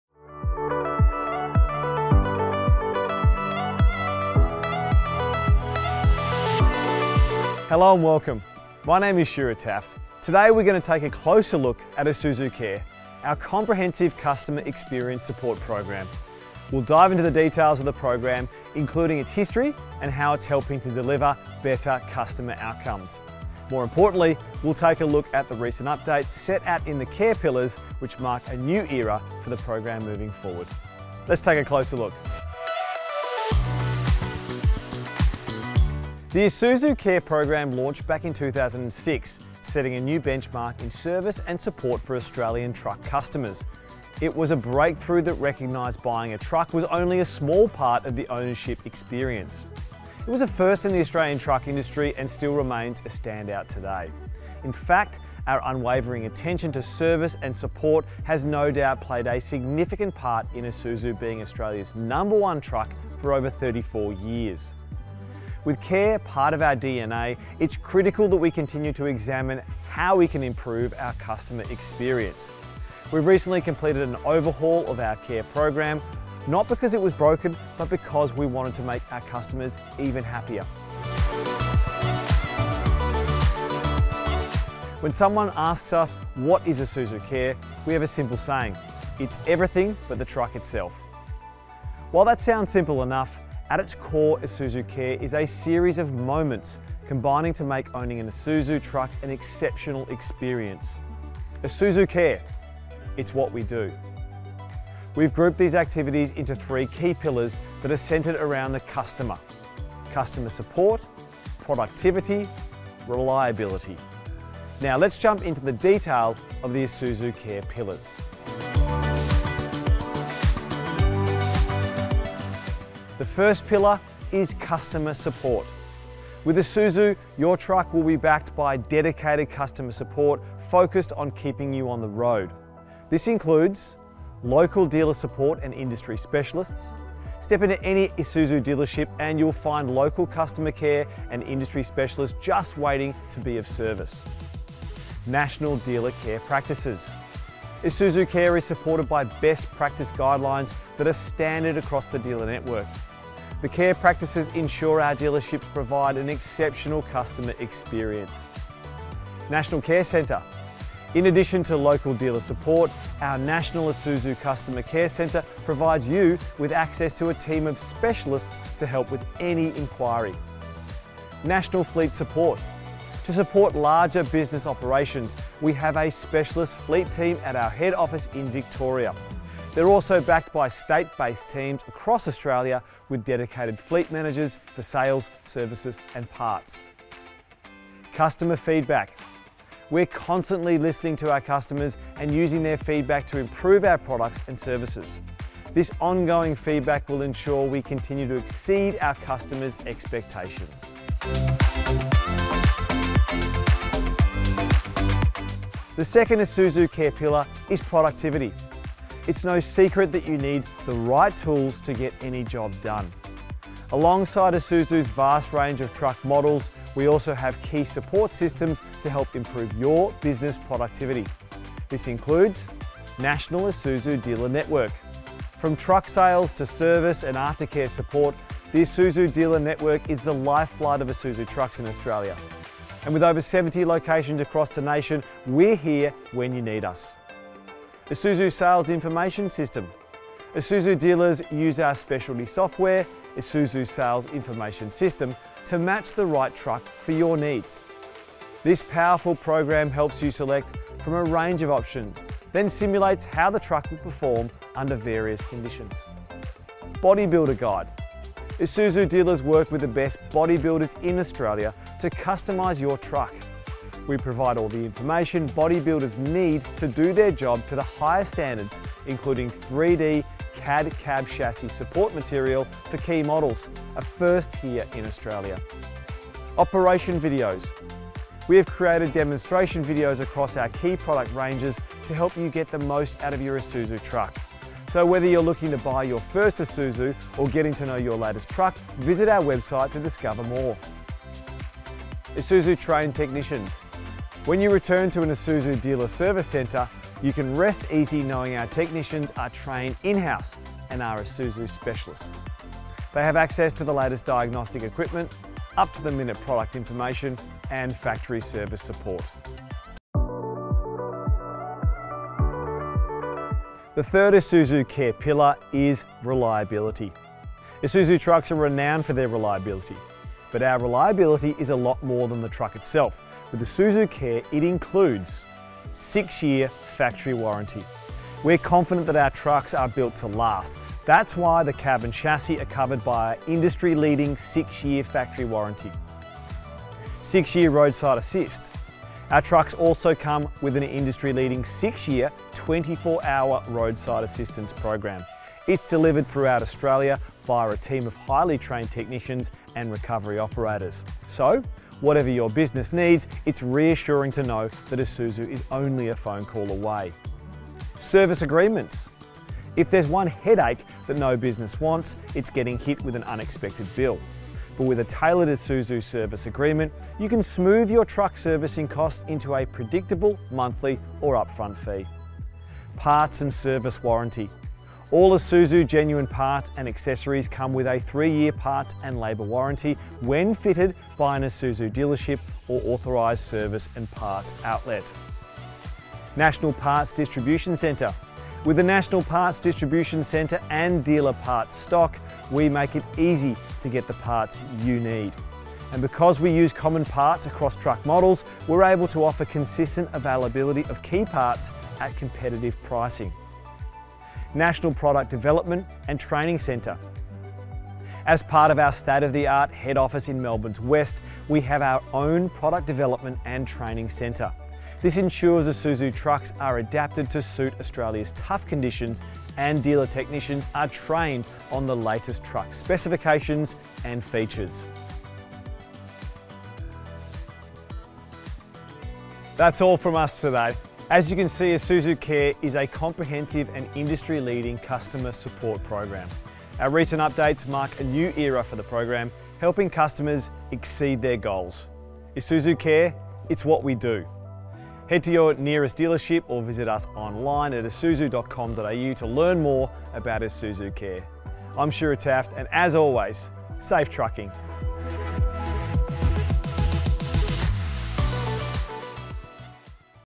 Isuzu Care - A closer look.
Join Shura Taft as he takes a closer look at Isuzu Care. Isuzu's comprehensive customer experience support program.

Care. It's what we do.